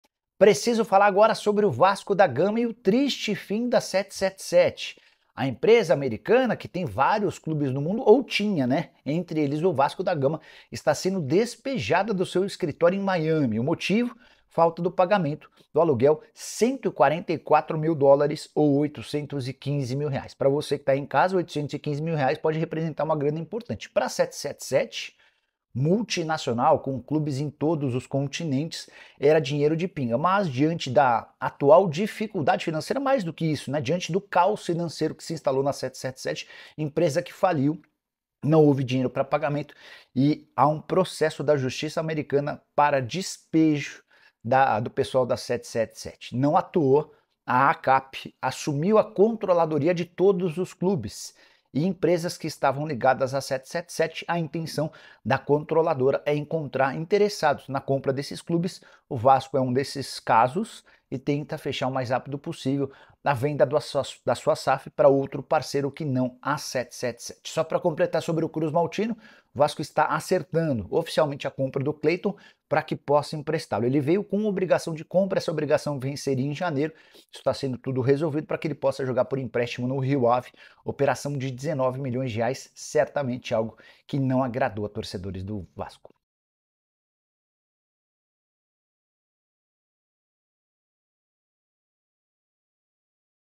TORCEDOR DO VASCO, A 777 ESTÁ ACABANDO!
Bem-vindo ao canal de Cortes do Nicola! Fala, galera! NESTE CORTE, vamos discutir o impacto da 777 no Vasco e como as coisas estão mudando. Confira as últimas atualizações e o que esperar para o futuro do clube. Não perca!